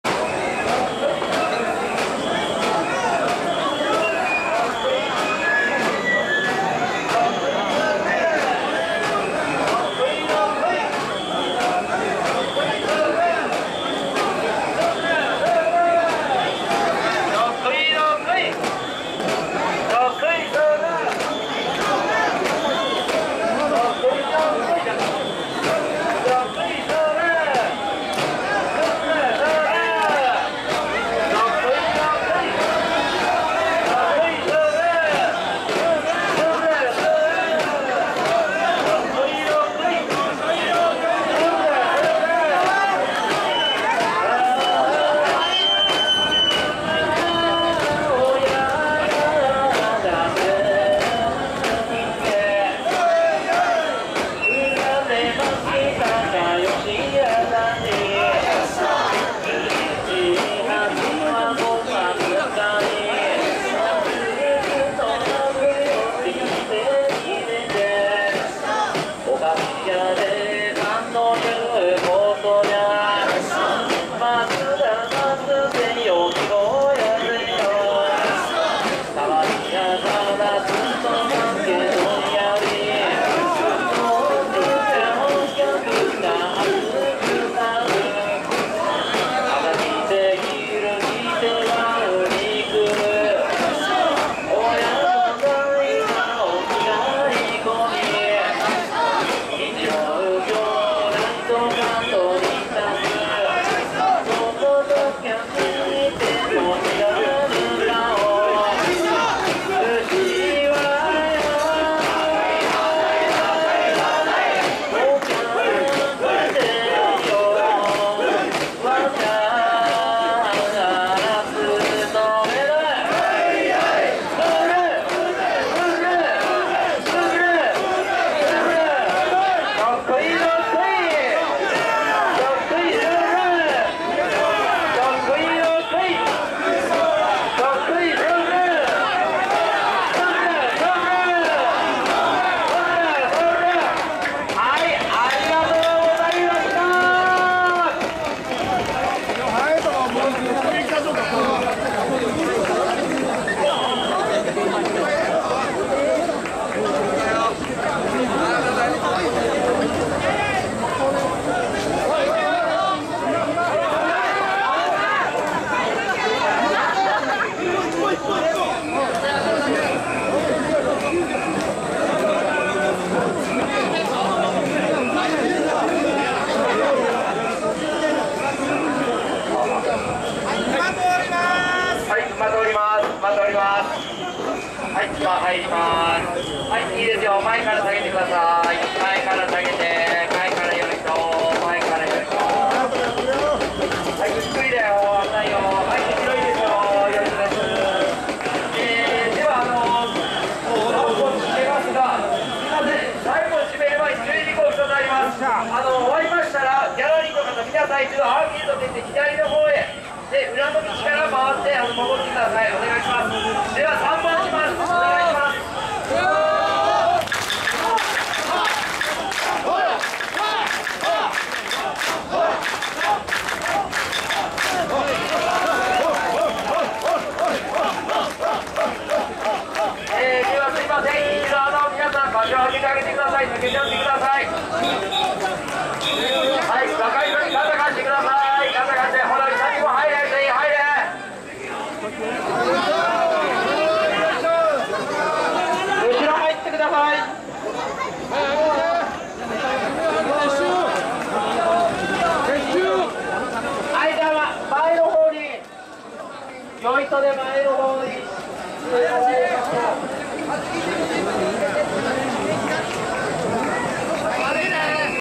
平成27年 横浜 開港祭 御輿コラボレーション「 神輿ﾊﾟﾚｰﾄﾞ」。
【キューバとジャパン文化交流】です。横浜 開港祭 神輿コラボレーション。伊勢佐木町　伊勢崎モール。　神輿18基　コラボレーション。
担ぎ手の皆さん　お疲れ様でした。
伊勢佐木　地名の由来。　 横浜市中区にある伊勢佐木町は、出来たのは新しく横浜開港から14年目の明治7年（1875）です。 1丁目～7丁目の約1.2km続く商店街が中心です。 明治時代この地の開発に力のあった伊勢屋中村治郎兵衛、佐川儀右衛門、佐々木新五郎の3人の姓から、 それぞれ「伊勢」「佐」「木」をとったものと言われている。 もとは入江を埋めた吉田新田の北一つ目沼地で、寛文11年（1671）までには埋め立てられて耕地であった。 明治2年（1869）には当地付近に港崎遊郭ができ、商店が立ち並び町の基礎ができ繁華街へ発展する。 よく誤解される群馬県は「伊勢崎」